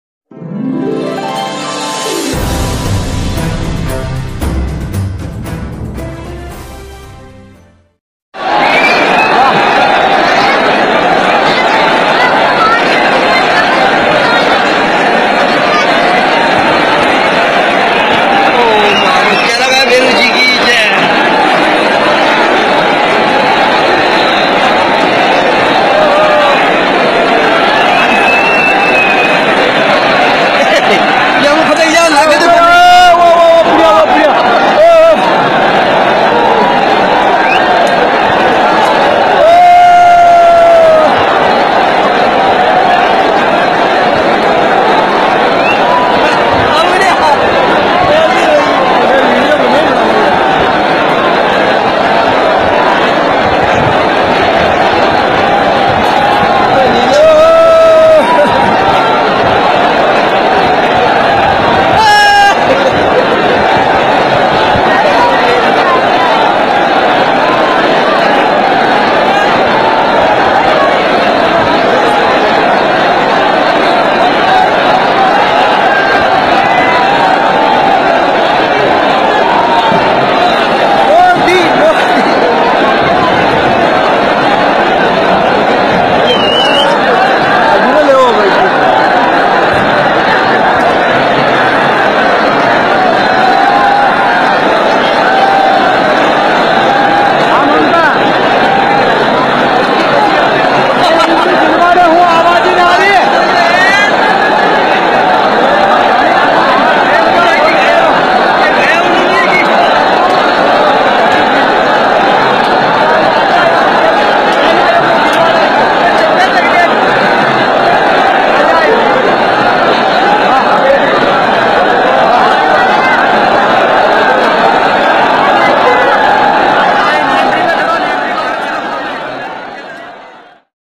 जोधपुर में पीएम मोदी के सबसे बड़े फैन, देखिए पूरा जनसैलाब
Welcome public poltics | news | poltics news | public poltics